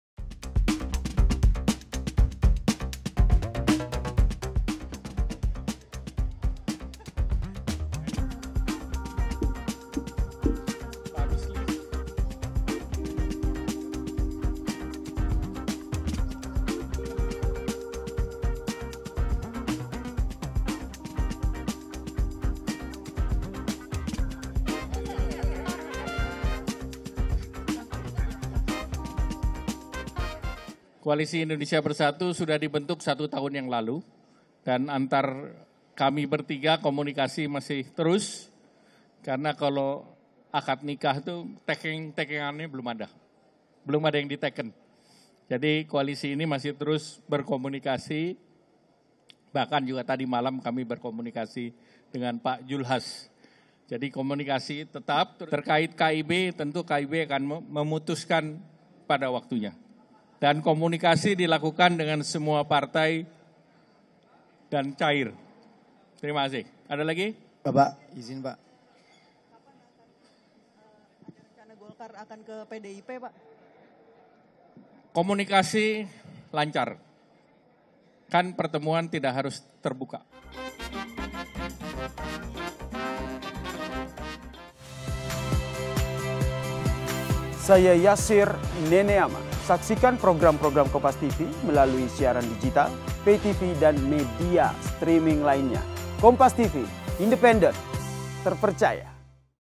Ketum Golkar Angkat Bicara soal Nasib Koalisi dengan PAN dan PPP
JAKARTA, KOMPAS.TV - Ketua Umum Partai Golkar, Airlangga Hartarto, membicarakan nasib Koalisi Indonesia Bersatu.

Airlangga menyatakan bahwa komunikasi antara Partai Golkar, PAN, dan PPP masih berjalan dengan baik.

"Koalisi Indonesia Bersatu sudah dibentuk 1 tahun yang lalu dan antar kami bertiga komunikasi masih terus," ujar Airlangga Hartarto, Ketum Partai Golkar pada Minggu, (4/6/2023).

"Kalau akad nikah itu, belum ada yang taken, jadi koailisi ini masih terus berkomunikasi," lanjutnya.

Saat ditanya media apakah Golkar akan bergabung ke PDIP, Airlangga hanya menjawab singkat bahwa komunikasi lancar dan komunikasi tidak harus terbuka.

Diketahui memang PPP dan PAN mulai merapat ke PDIP.